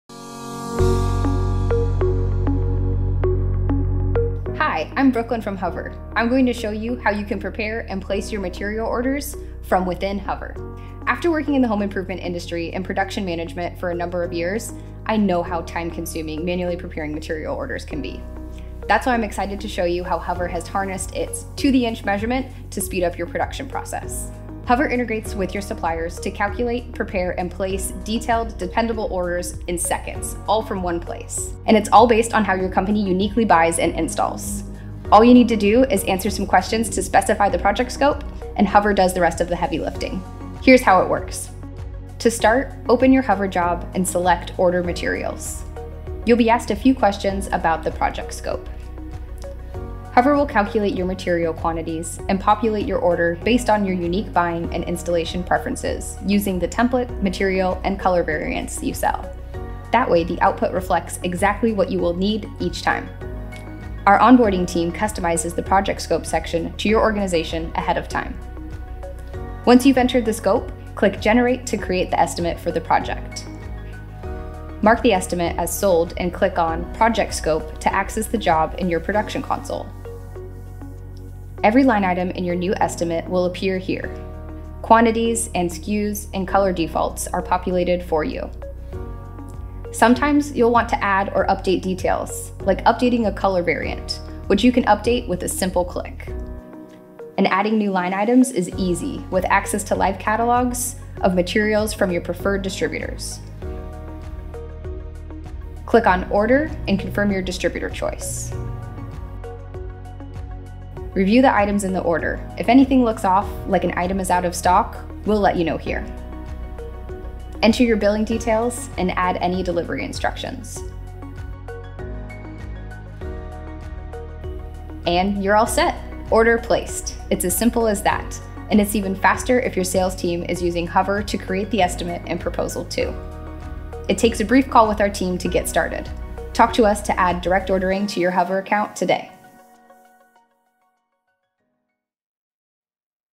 How to place material orders with your suppliers in HOVER
With Direct Ordering, you can calculate and submit your roofing material orders in just minutes—all in one place. This demo video shows the step-by-step for how any HOVER customer with Direct Ordering enabled can prepare and place an order.